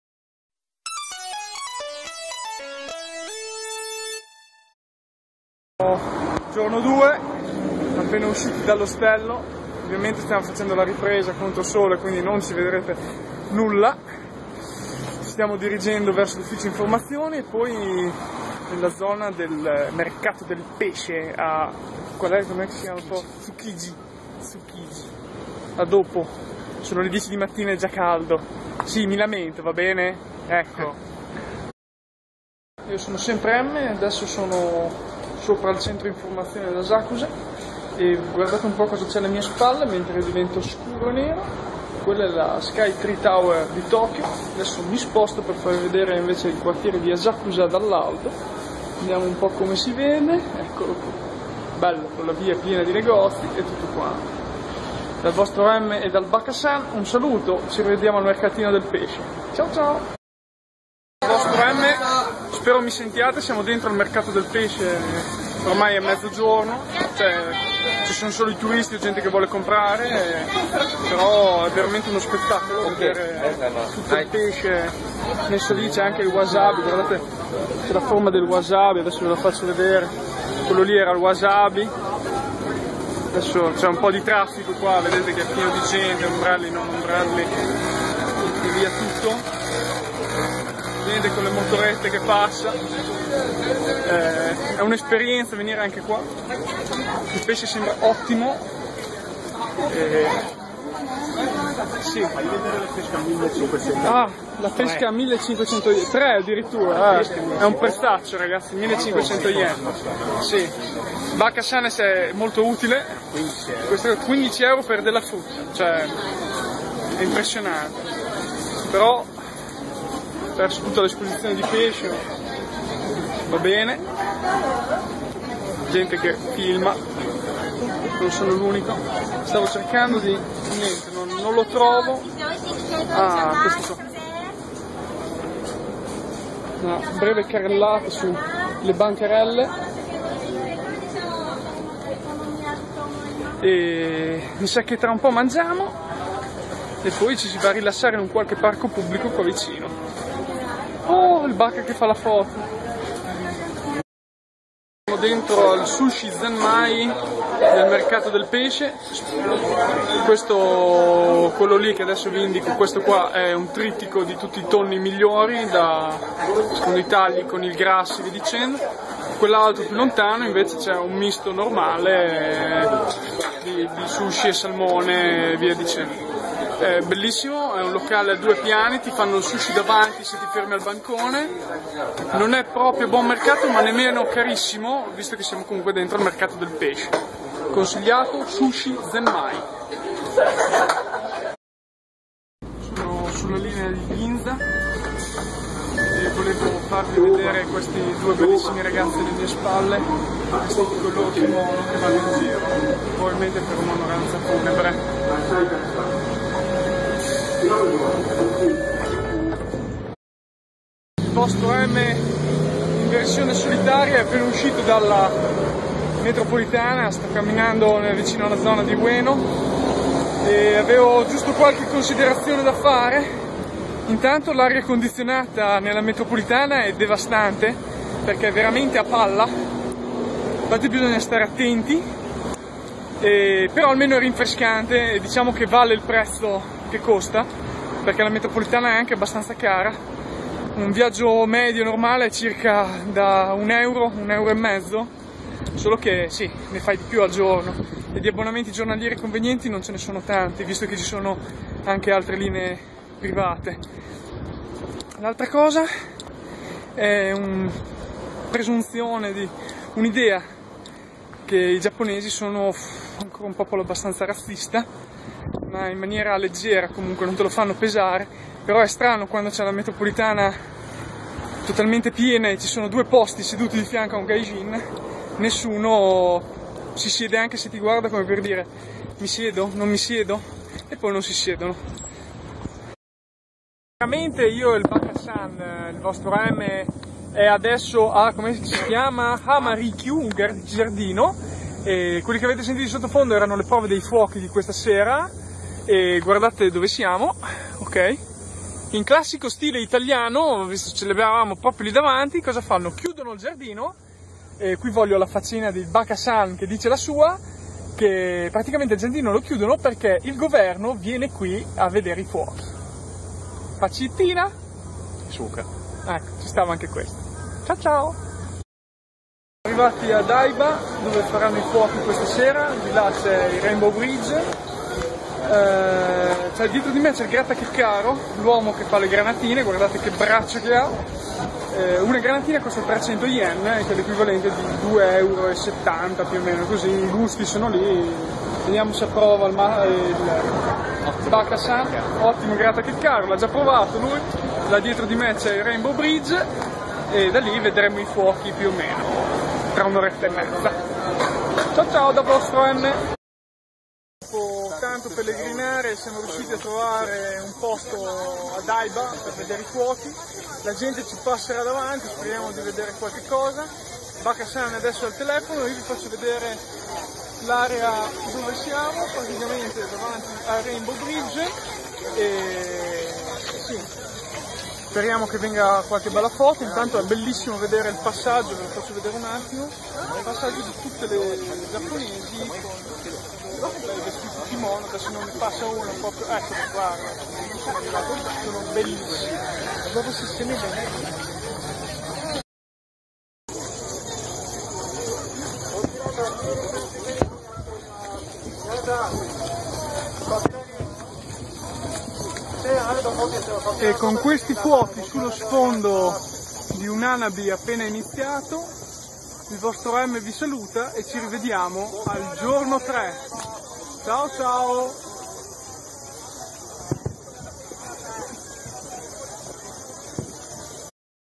MTE 10/08/2013 - Giappone Giorno 2
Una giornata passata tra il mercato del pesce e i fuochi d'artificio sulla baia di Tokyo.
Mi son lasciato andare pure a qualche considerazione personale sui giapponesi.